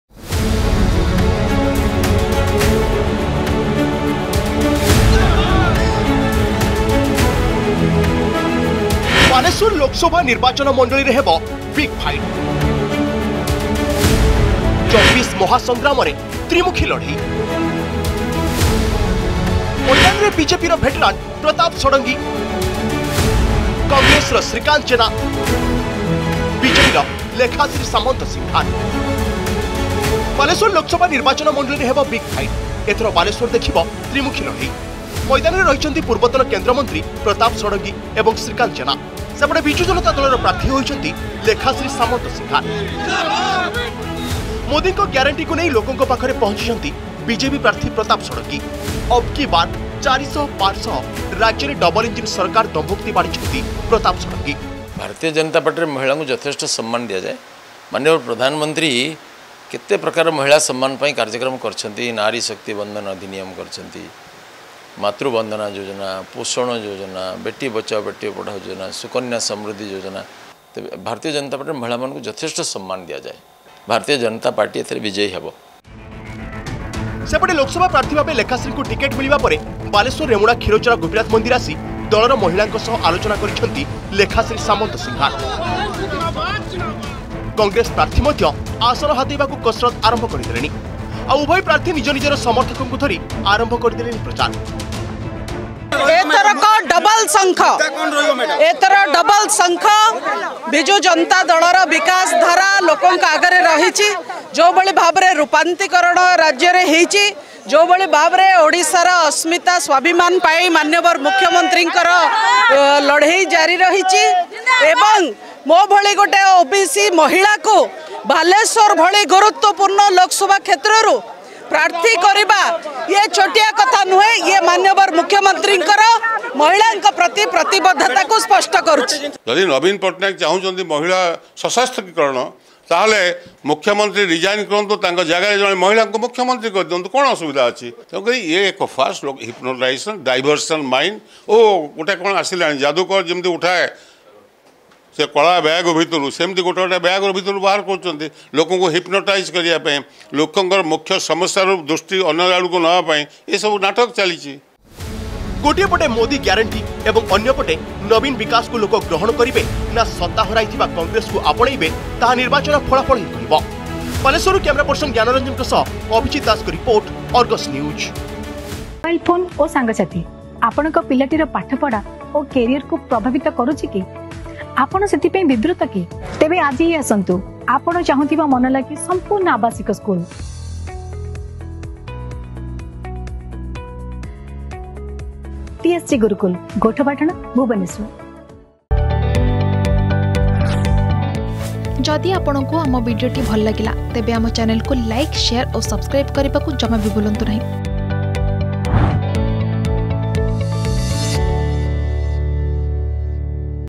ବାଲେଶ୍ଵର ଲୋକସଭା ନିର୍ବାଚନ ମଣ୍ଡଳୀରେ ହେବ ବିଗ୍ ଫାଇଟ୍ || 2024 Election || Balasore
ବାଲେଶ୍ଵର ଲୋକସଭା ନିର୍ବାଚନ ମଣ୍ଡଳୀରେ ହେବ ବିଗ୍ ଫାଇଟ୍। ଏଥର ବାଲେଶ୍ବର ଦେଖିବ ତ୍ରିମୁଖୀ ଲଢେଇ । ମଇଦାନରେ ରହିଛନ୍ତି  ପୂର୍ବତନ କେନ୍ଦ୍ର ମନ୍ତ୍ରୀ ପ୍ରତାପ ଷଡଙ୍ଗୀ ଓ ଶ୍ରୀକାନ୍ତ ଜେନା  ସେପଟେ ବିଜୁ ଜନତା ଦଳର ପ୍ରାର୍ଥୀ ହୋଇଛନ୍ତି ଲେଖାଶ୍ରୀ ସାମନ୍ତସିଂହାର।